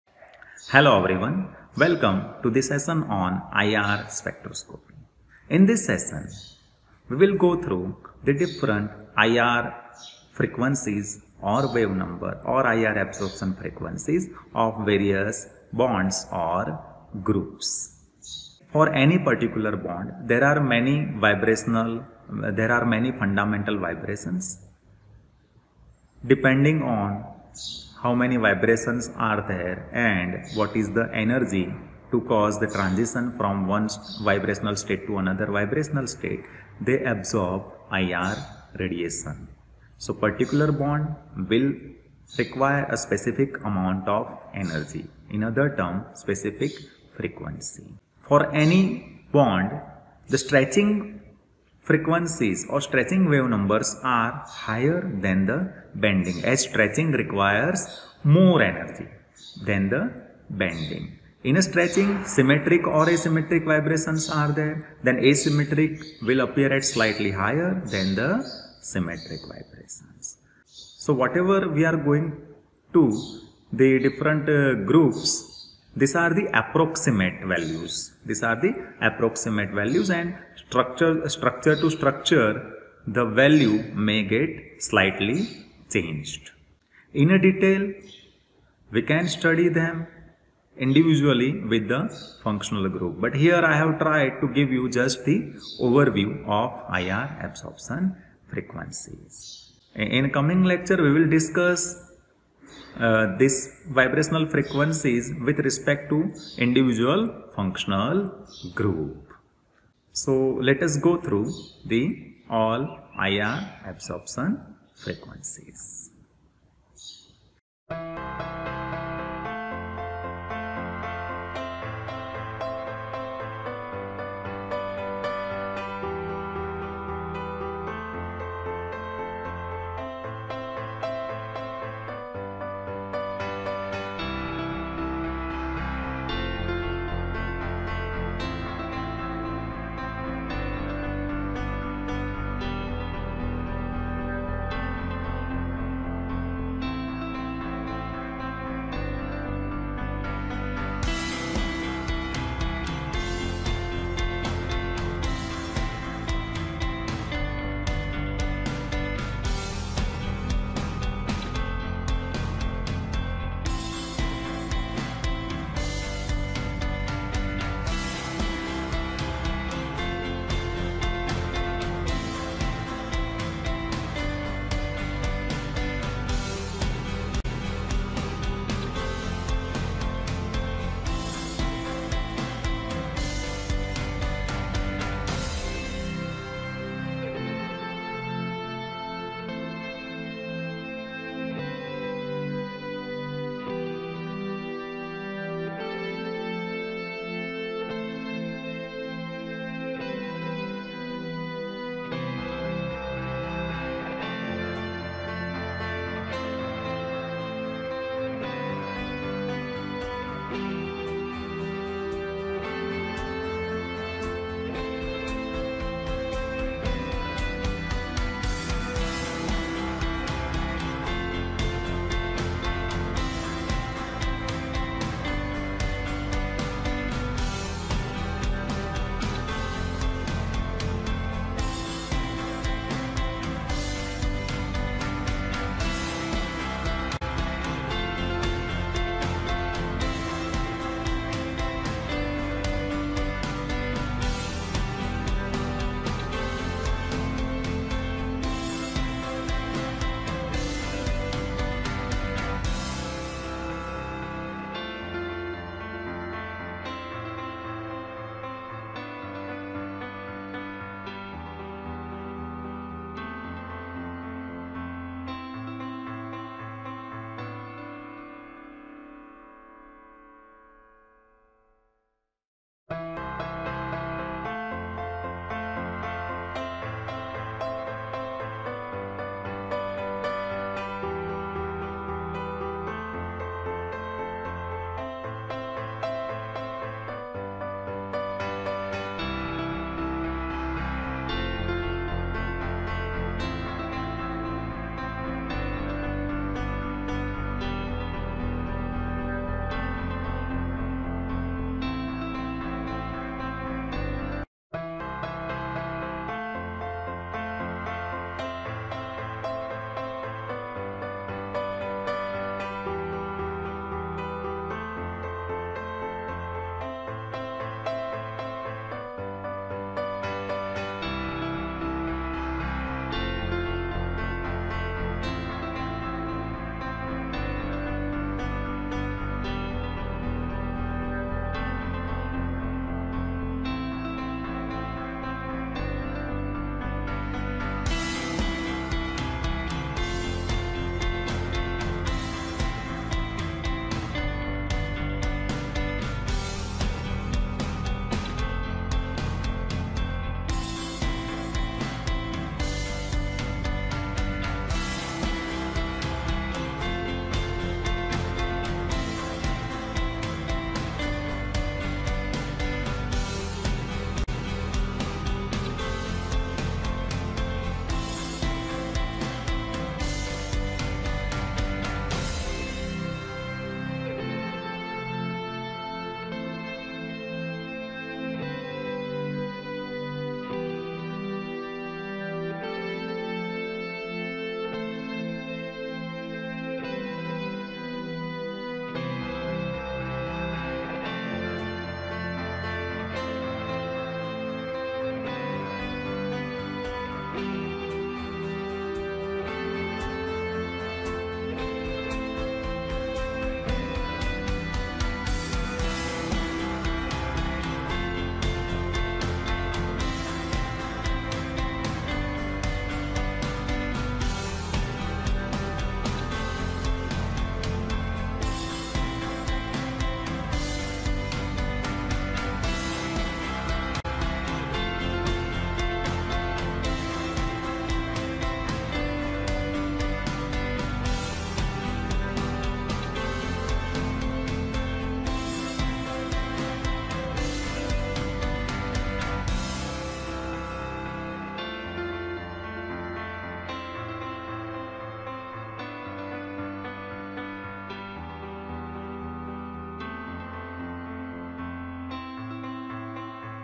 Infrared Spectroscopy (IR Spectroscopy ) Part-4 IR frequencies / Wave numbers | functional groups
IR Spectroscopy - Analytical Technique  in Analytical Chemistry
Vibration Frequencies
Absorption frequencies of Organic Functional groups:

benzene, stretching and bending

Infrared Spectroscopy is Analytical Technique used for Analysis of Organic compounds with functional groups. IR spectroscopy is used to determine IR or infrared absorption frequencies or vibration frequency of stretching and bending vibrations in IR spectroscopy. The technology is useful for the students of Pharmacy, Analytical Chemistry and Organic Chemistry.It is useful for the competative exams like GPAT, NIPER-JEE, GATE, CSIR NET.

Thanks,
Parixit Prajapati